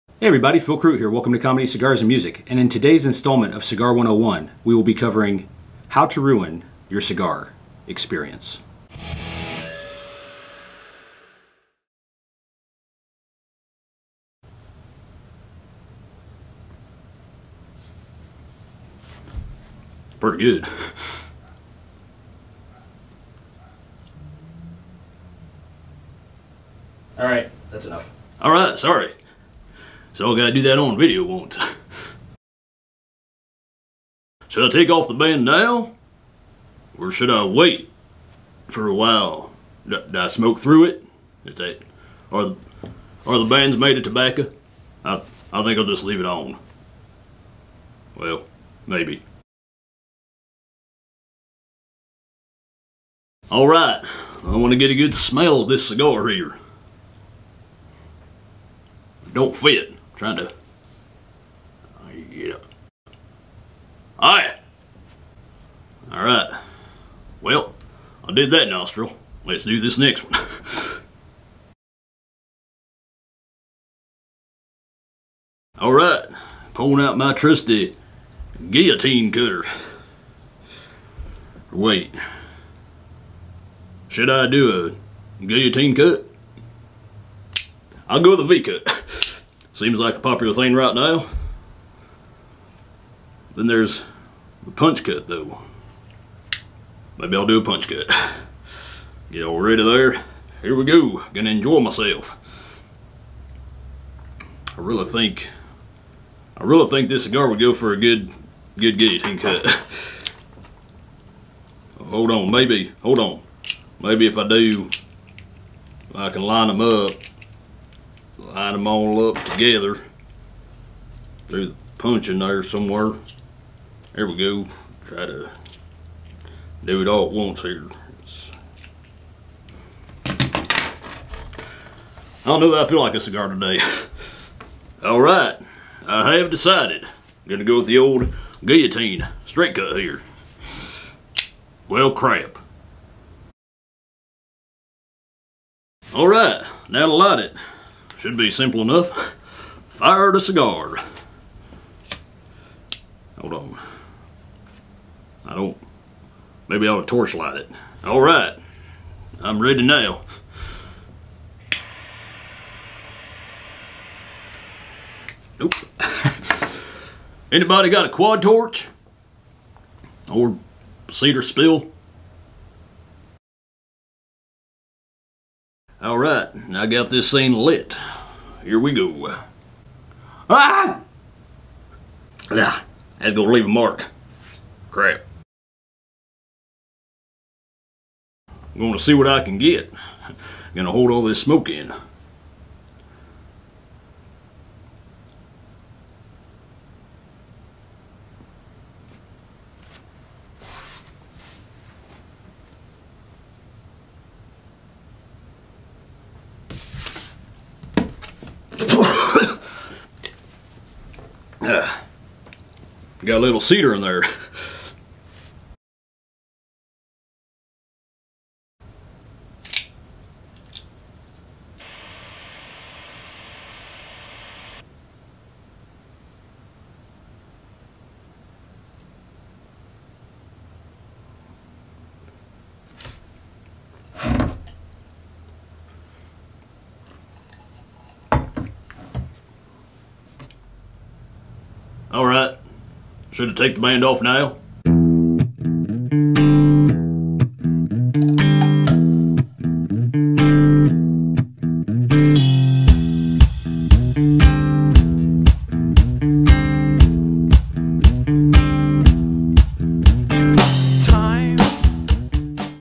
How To Ruin Your Cigar Experience | Cigar 101
In today's installment of Cigar 101, we cover How To Ruin Your Cigar Experience.

Copyright remains with the original songwriter(s) and/or artist(s).